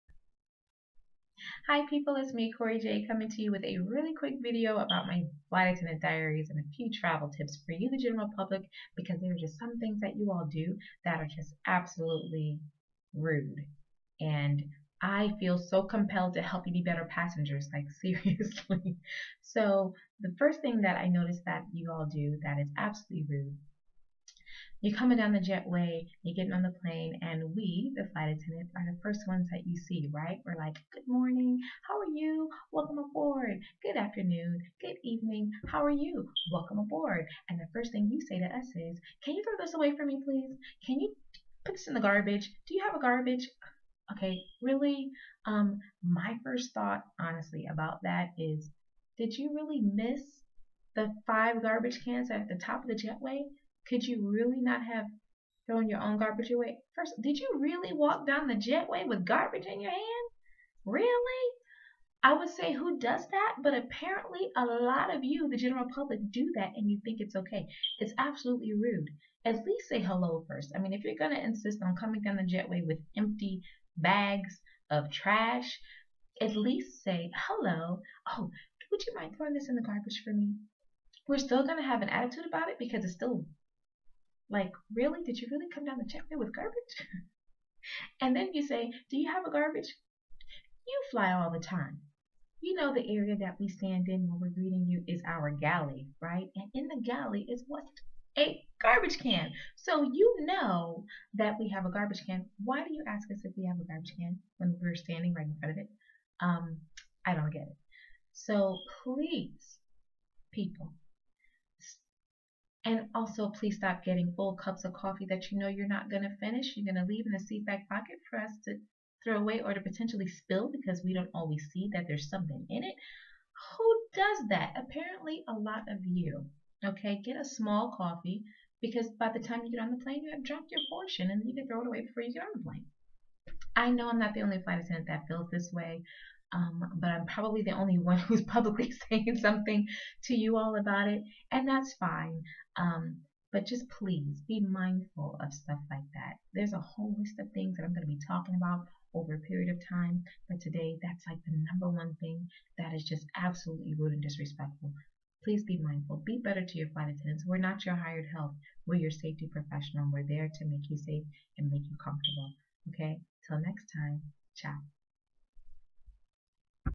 Flight Attendant Diaries
A few simple tips to help you become a better passenger! A must watch.